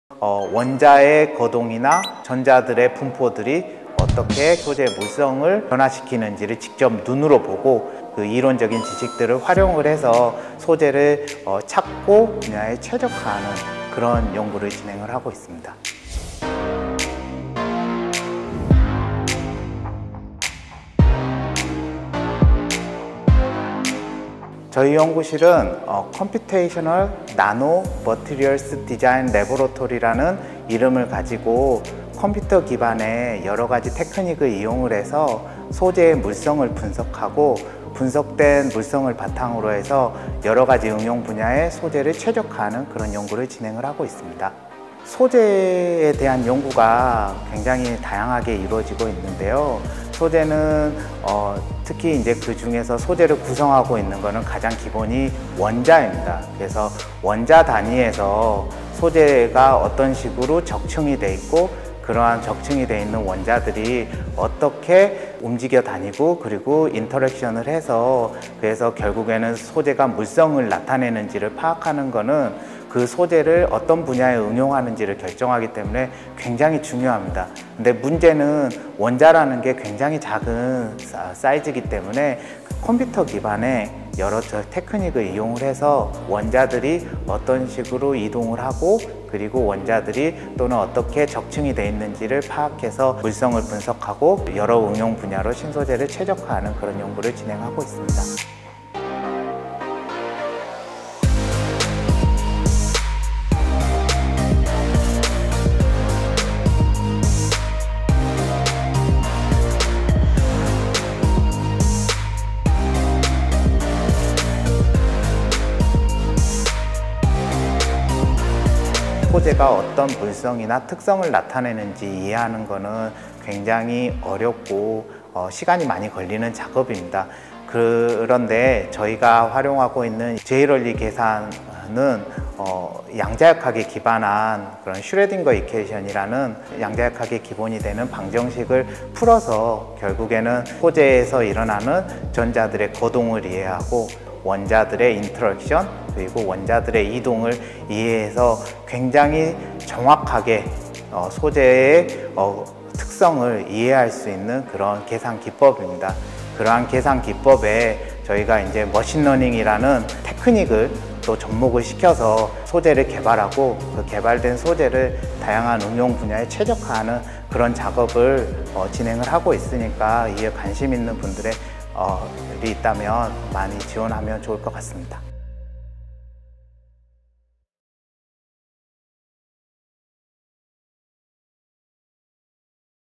⛓ 새로운 응용분야 개척하는 신소재, 전자 움직임 계산해 찾는다
계산으로 소재의 물성을 알아내면 수많은 실험을 통해 하나의 소재를 찾아내는 인고의 시간을 줄일 수 있다.  포스텍 전산나노소재(CNMD) 연구실은  기에 만족하지 않고 더욱 효율적으로 빠르게 소재 성능을 밝혀내는 연구를 진행하고 있다.